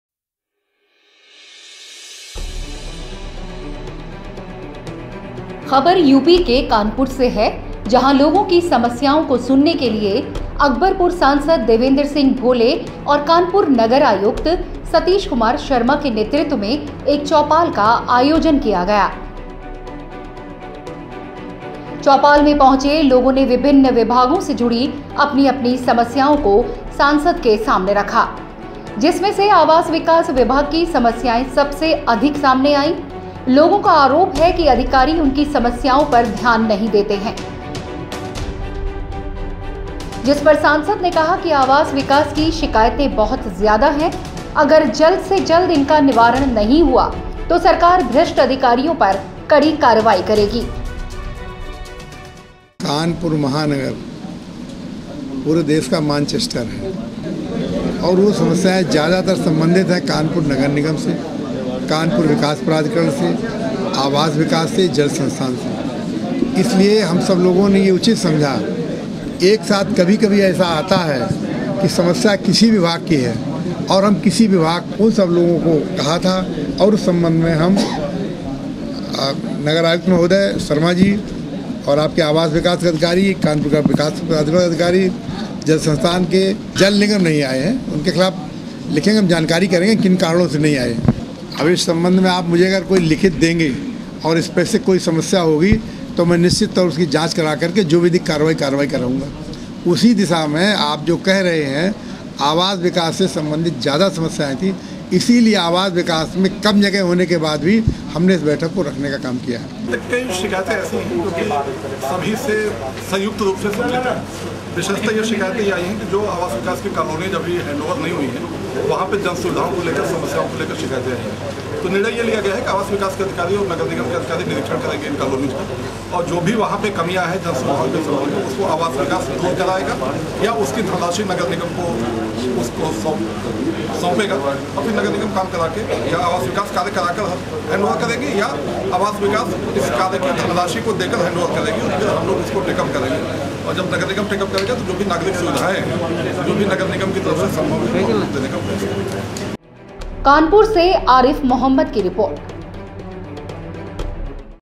MP devender bhole अधिकारियों के रवैये से नाराज हुए , कही ये बड़ी बात...
कानपुर से है जहां लोगों की समस्याओं को सुनने के लिए अकबरपुर सांसद देवेंद्र सिंह भोले और कानपुर नगर आयुक्त सतीश कुमार शर्मा के नेतृत्व में एक चौपाल का आयोजन किया गया... चौपाल में पहुंचे लोगों ने विभिन्न विभागों से जुड़ी अपनी-अपनी समस्याओं को सांसद के सामने रखा...